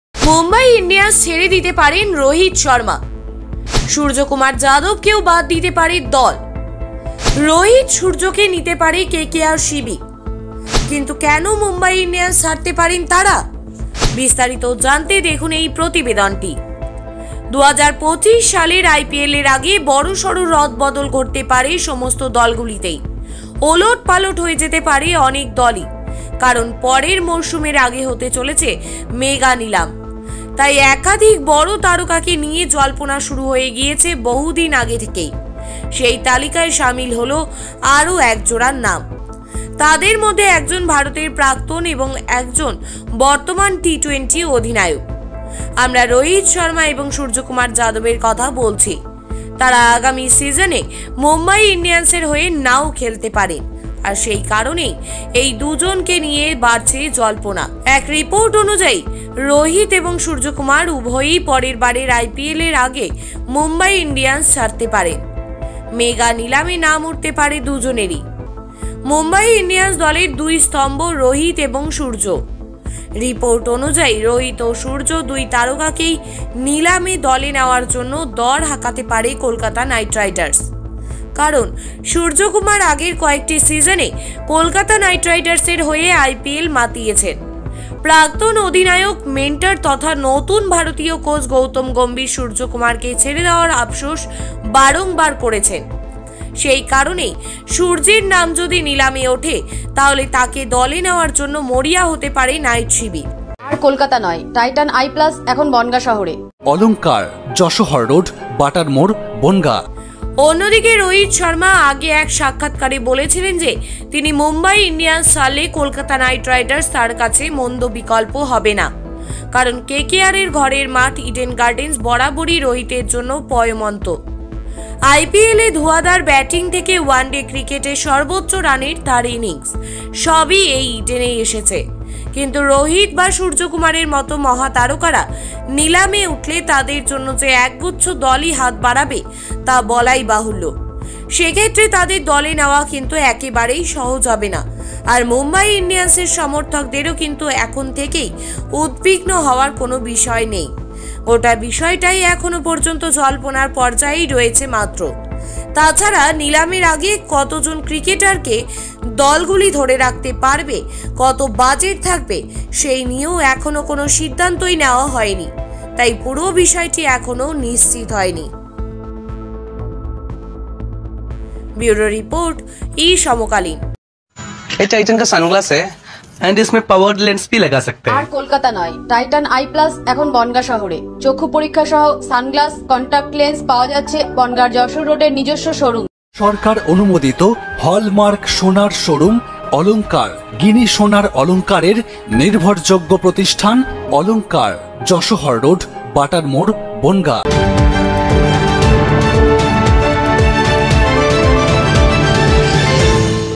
কেকেআর শিবিরে রোহিত-সূর্যকুমার? - Rohit-Suryakumar in KKR camp?
কেকেআর শিবিরে রোহিত-সূর্যকুমার?‌
Rohit-Suryakumar in KKR camp?

২০২৫ সালের আইপিএলের আগে বড়সড় রদবদল ঘটতে পারে সমস্ত দলগুলিতেই। ওলট-পালট হয়ে যেতে পারে অনেক দলই। কারণ, পরের মরশুমের আগে হতে চলেছে মেগা নিলাম। তাই একাধিক বড় তারকাকে নিয়ে জল্পনা শুরু হয়ে গিয়েছে বহুদিন আগে থেকেই। সেই তালিকায় সামিল হল আরও একজোড়া নাম। তাঁদের মধ্যে একজন ভারতের প্রাক্তন এবং একজন বর্তমান টি-টোয়েন্টি অধিনায়ক। আমরা রোহিত শর্মা এবং সূর্যকুমার যাদবের কথা বলছি। তাঁরা আগামী সিজনে মুম্বই ইন্ডিয়ান্সের হয়ে নাও খেলতে পারেন। আর সেই কারণেই এই দু'জনকে নিয়ে বাড়ছে জল্পনা।